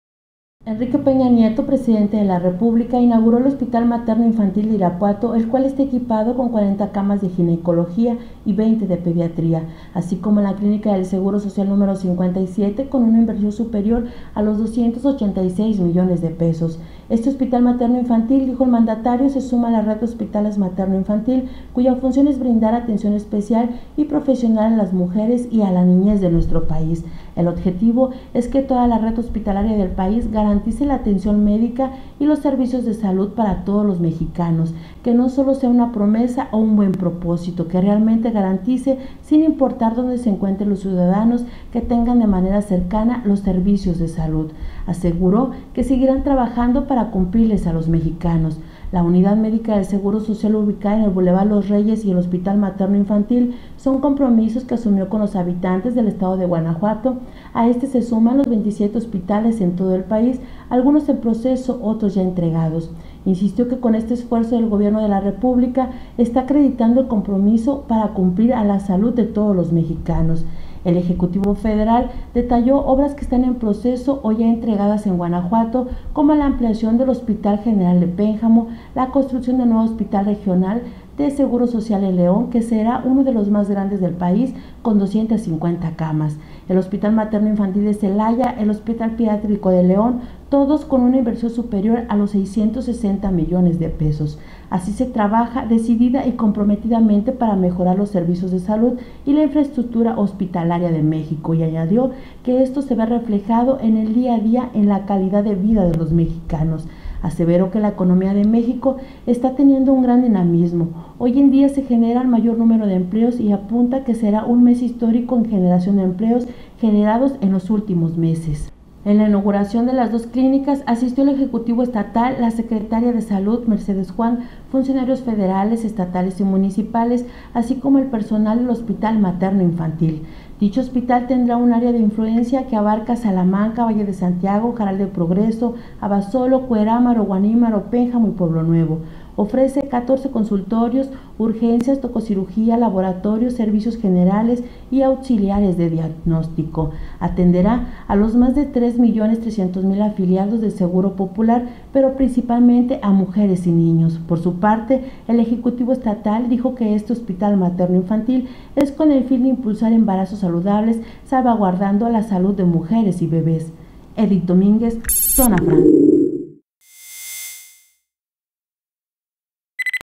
Inauguración del Hospital Materno Infantil de Irapuato
Enrique Peña Nieto, presidente de la República, inauguró el hospital Materno Infantil de Irapuato, el cual está equipado con 40 camas de ginecología y 20 de pediatría.